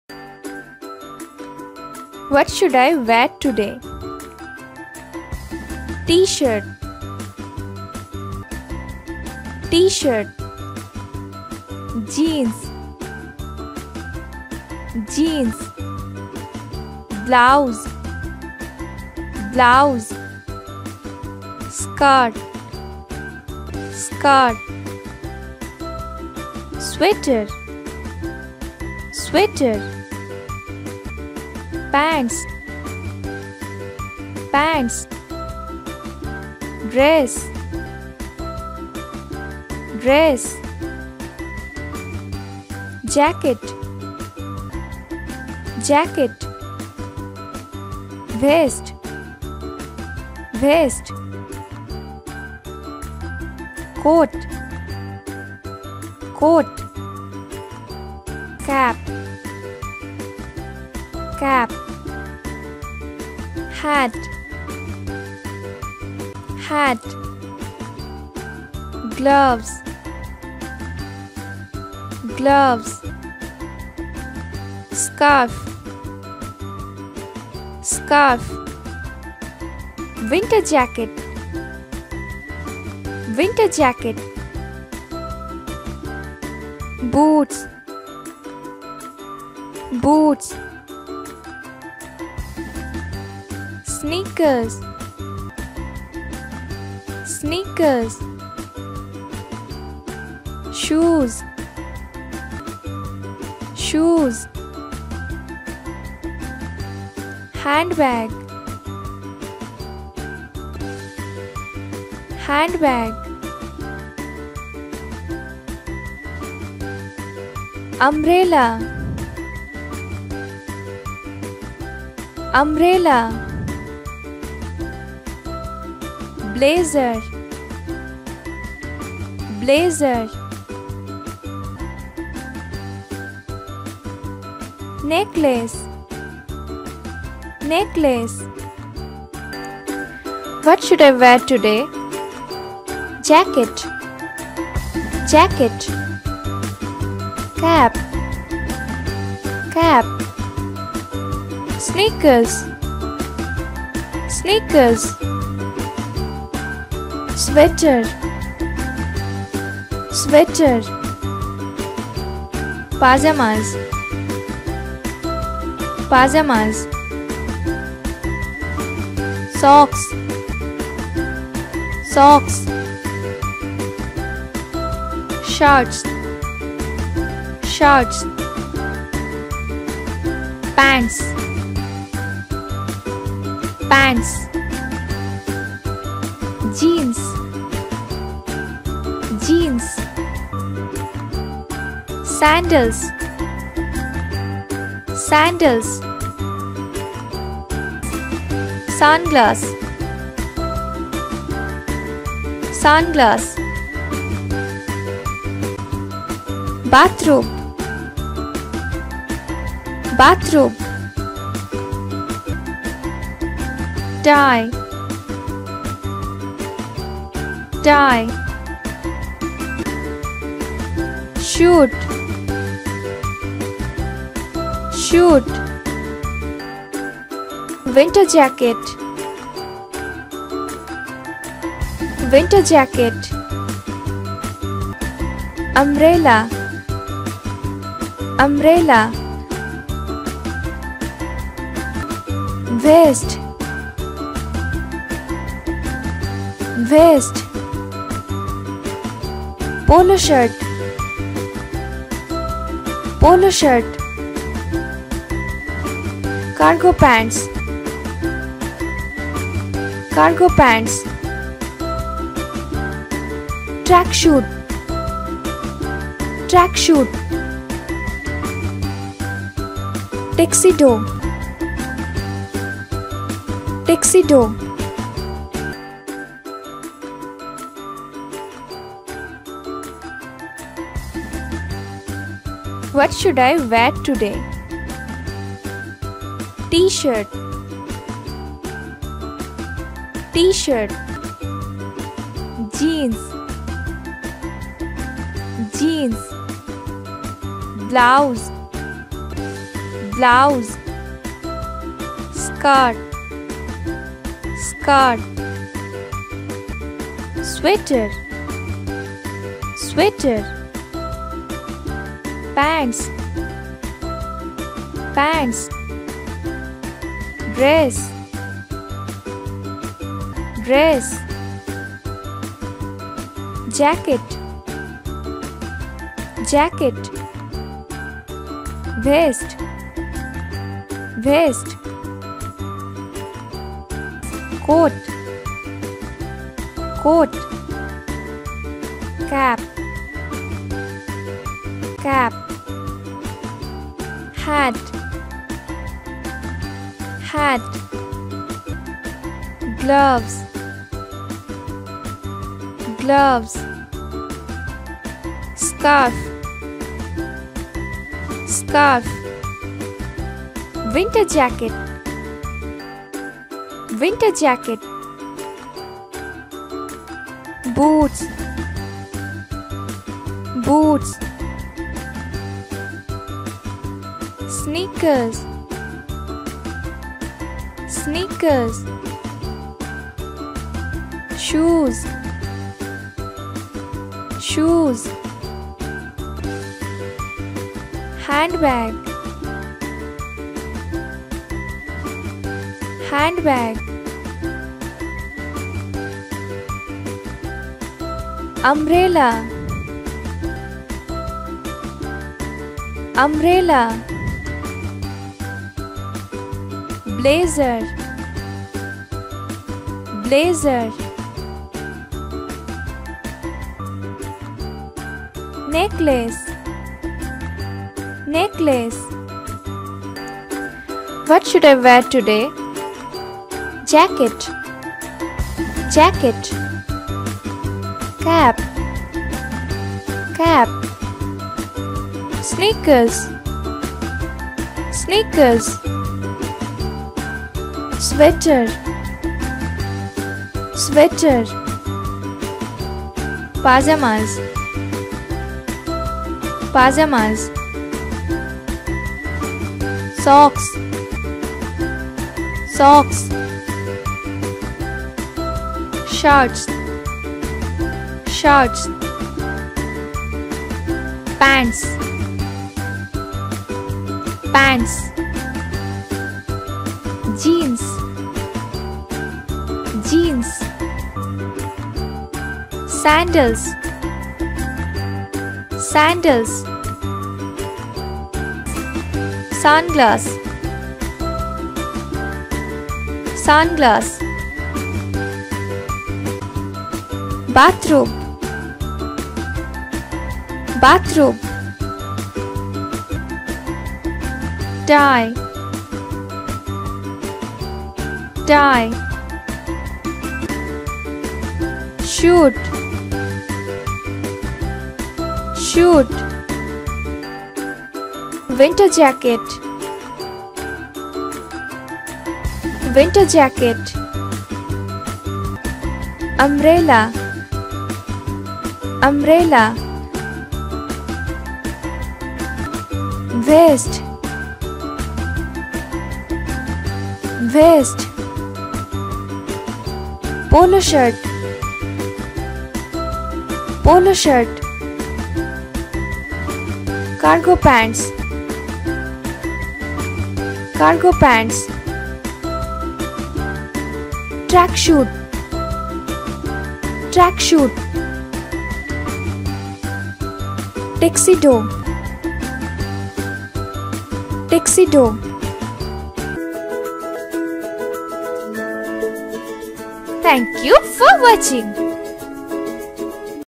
Çocuklar için İngilizce Kelimeler - Kıyafetler
Çocuklar için ezberlenmesi gereken İngilizce kelimeler videosu.
Bu videoda bilmemiz gereken kıyafet isimlerini öğreneceğiz.
Altyazıları göremiyorsanız "tam ekran" tuşunun yanındaki "CC" yazısını tıklayın.

I Love Creative Commons!